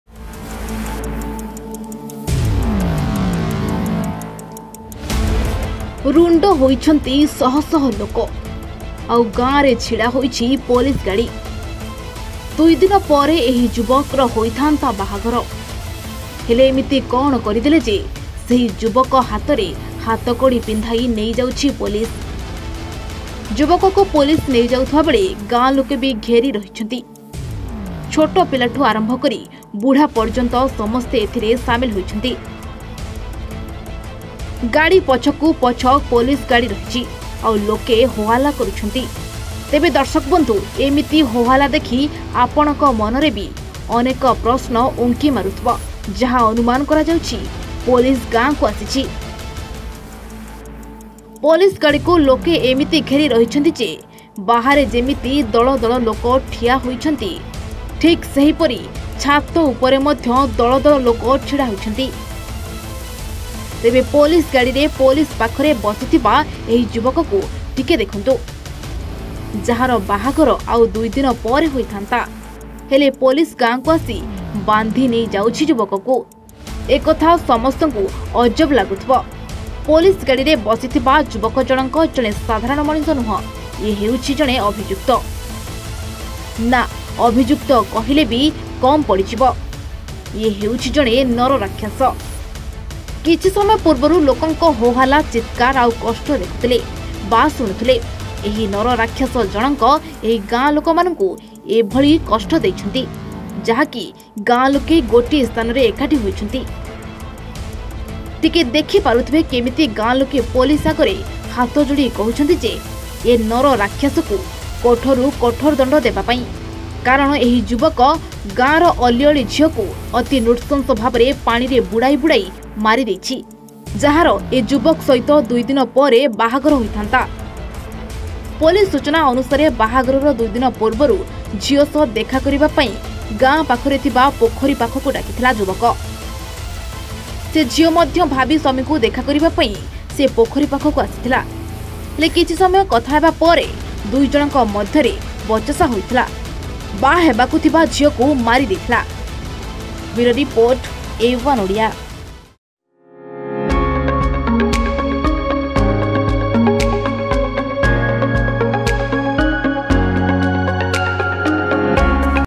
ଯାହାକୁ ବାହାହେଇଥାନ୍ତା ତାକୁ ଦେଖାକରିବାକୁ ଡାକି ପୋଖରୀ ଭିତରେ ଜୀବନ ନେଇଗଲା l VIRAL VIDEO l @A1odia l
🔆MORE UDATE🔆